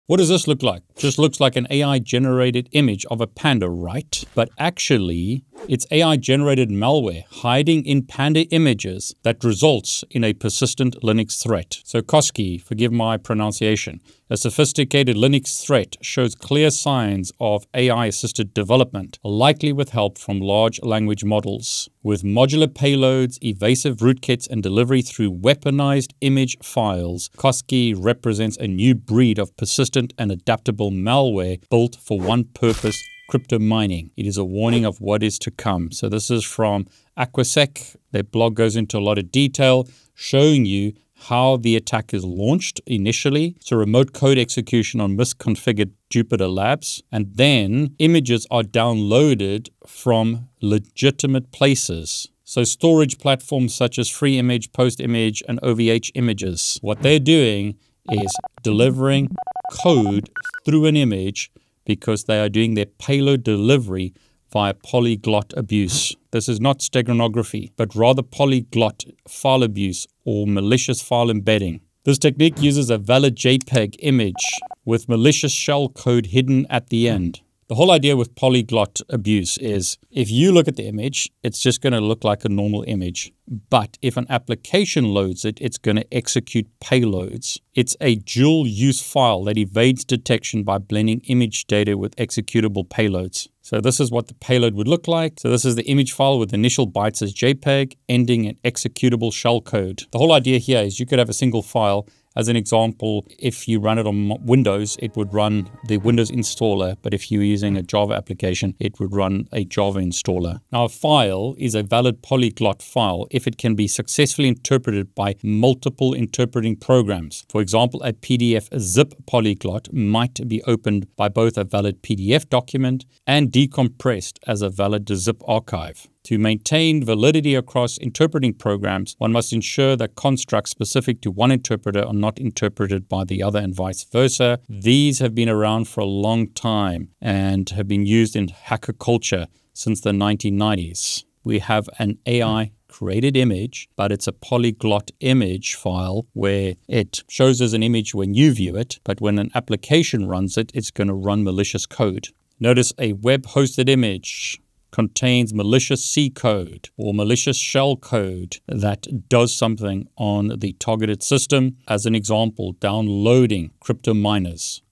DANGEROUS AI Malware Hides in JPEGs🚨
Discover Koske, a new breed of AI-built malware that hides in harmless-looking JPEGs and turns your Linux host into a silent crypto-miner. In this breakdown you’ll learn:

• The JupyterLab misconfiguration that gives attackers remote code execution
• Why polyglot file abuse beats classic steganography for sneaking in shellcode
• How its modular rootkit evades scans and persists after reboot
• Practical defences: hardening tips, hash-based scanning, and network egress rules
Stay ahead of image-based threats—watch now and lock down your servers.